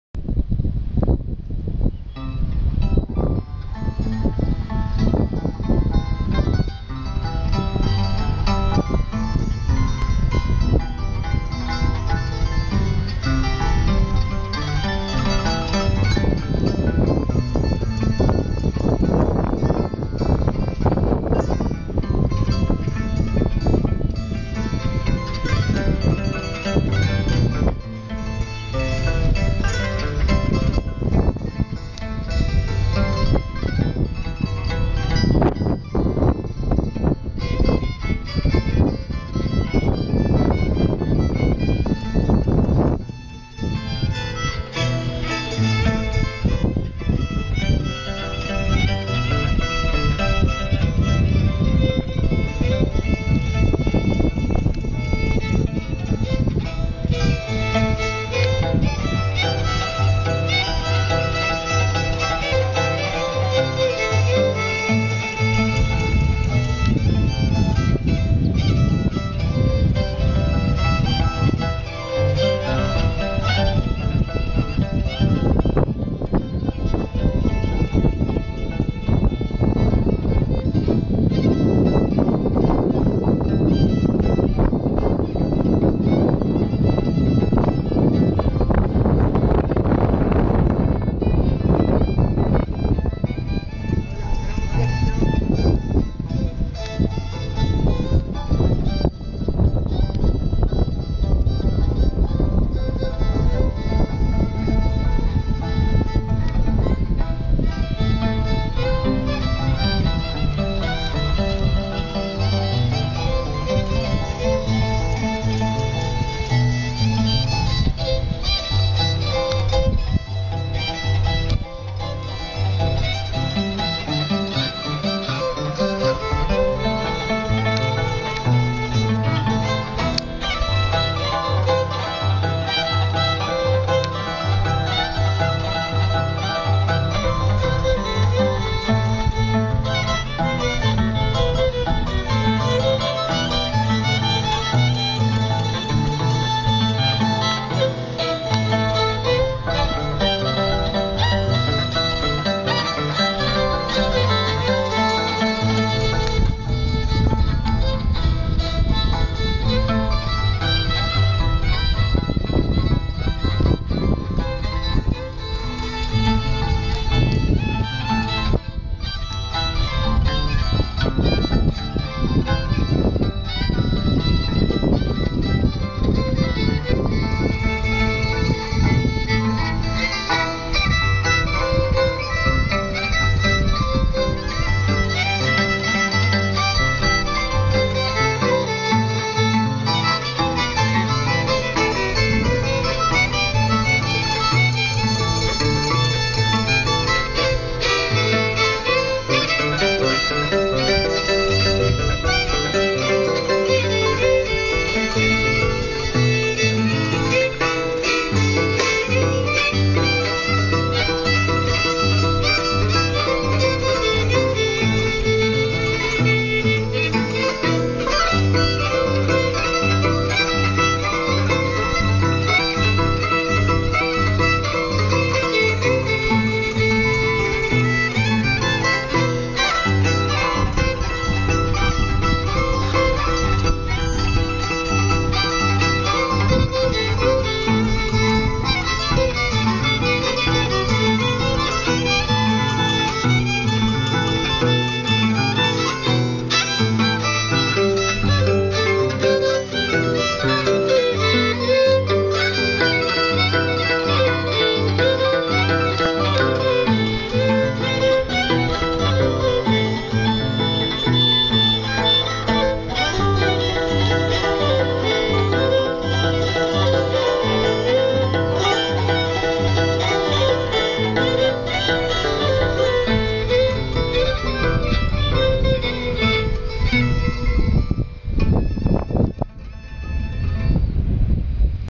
pacapauza (melecio y jair)
provincia de parinacochas - PERU
PACAPAUZA tocando con arpa y violin mostrando los bellos paisajes de pacapauza